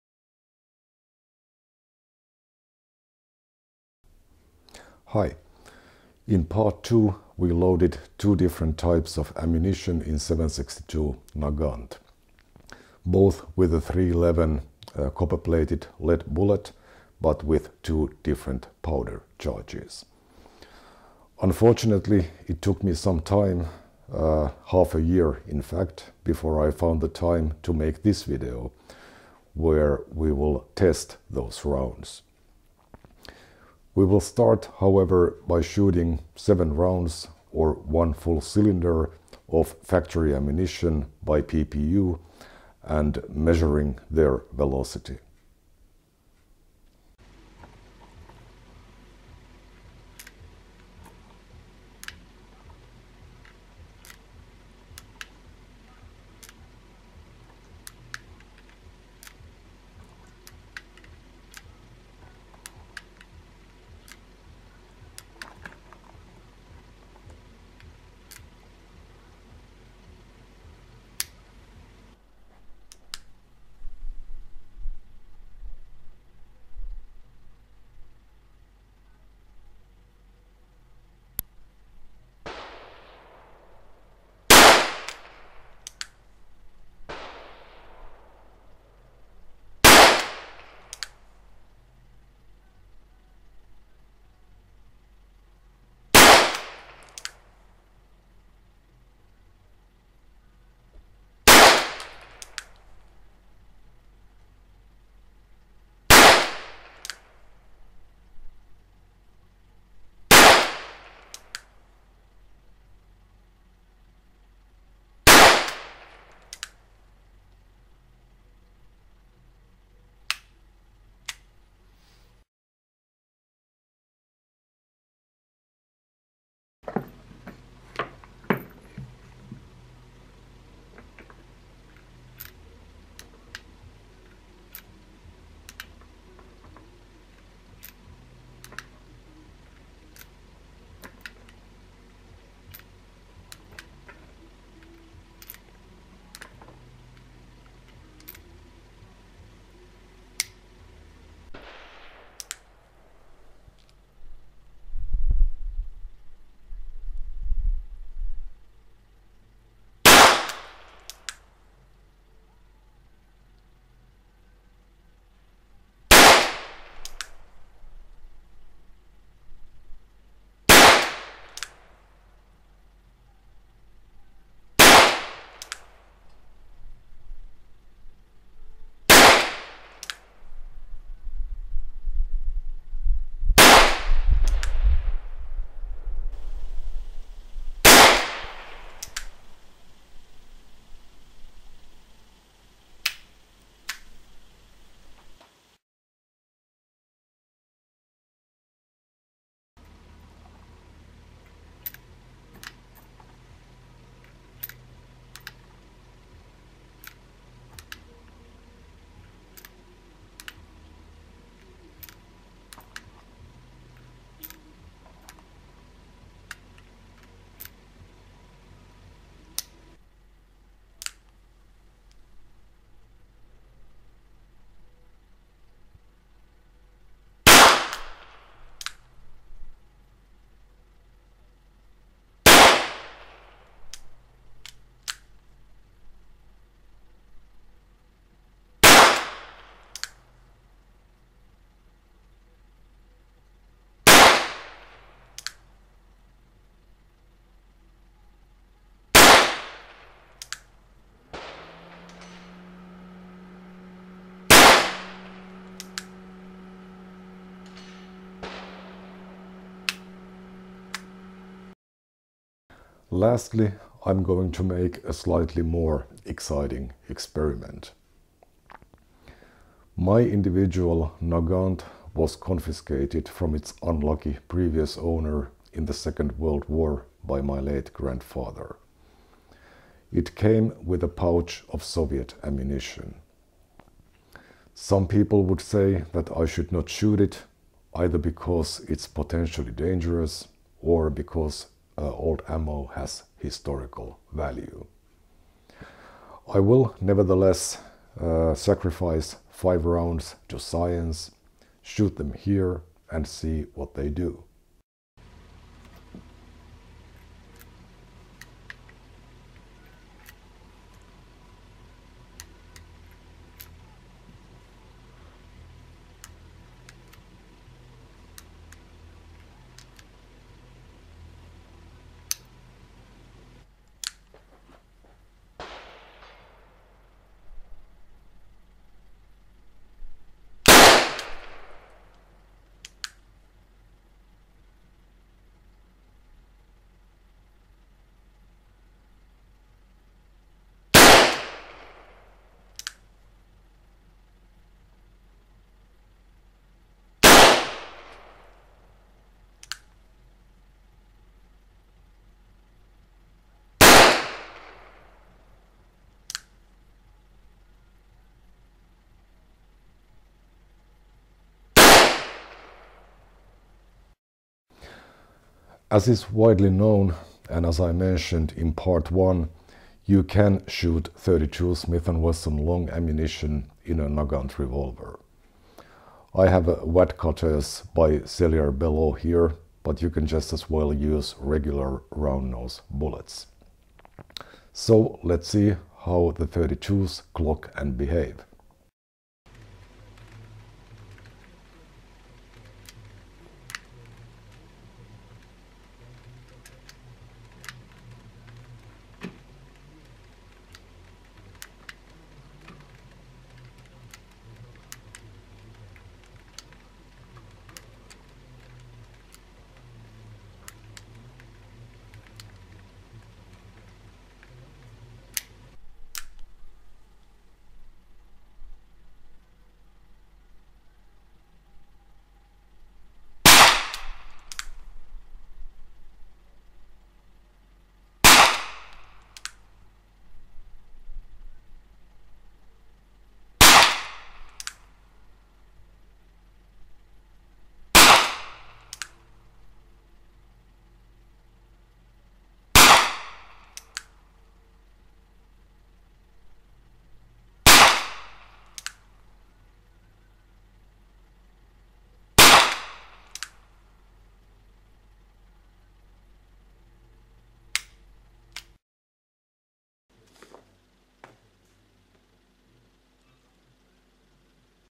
Reloading 7.62 Nagant, Part 3: At the range
Testing hand-loads, factory rounds by PPU, .32 S&W ammunition, and WW2 military ammunition with a Nagant revolver.

The chrono had difficulties with the cold weather and snow. I cannot guarantee that all the readings are absolutely reliable.